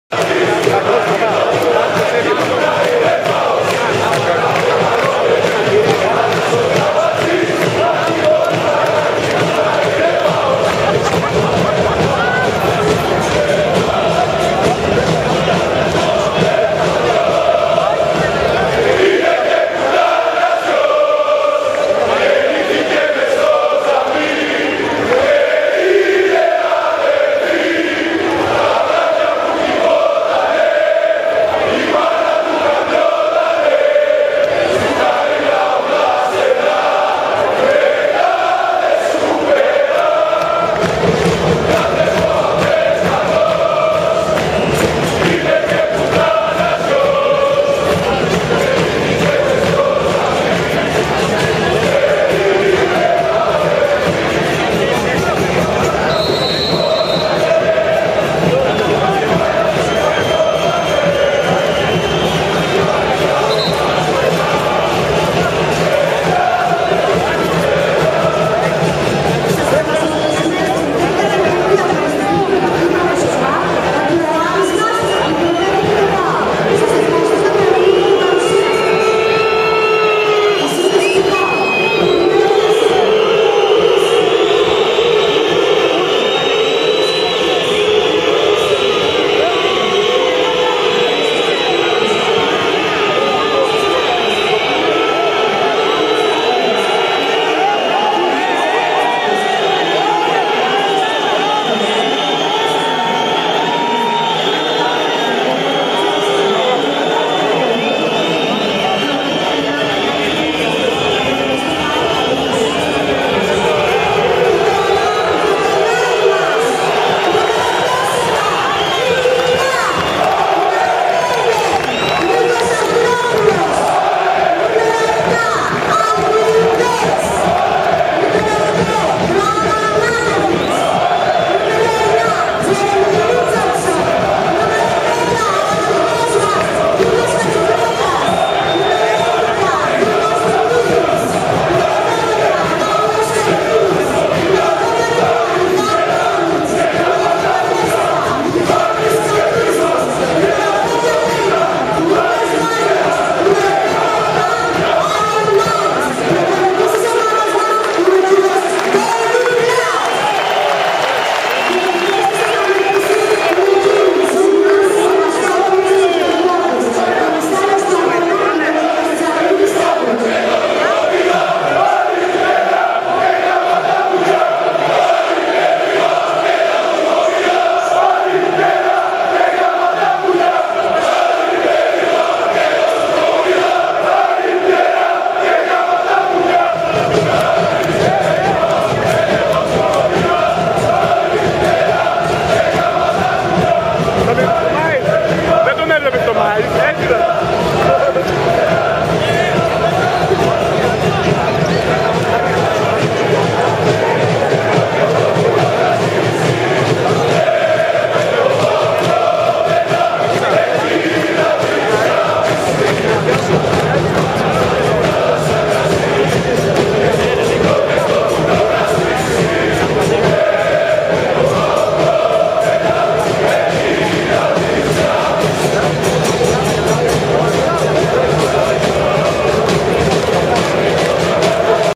Aris-paok basket 69-59 6/2/2010
Pre Game Show. 69-59. Poly oraia bradia, ekplhktikh atmosfaira, polys xabales apo 6000 Areianous pou de stamathsan na tragoudane oute lepto.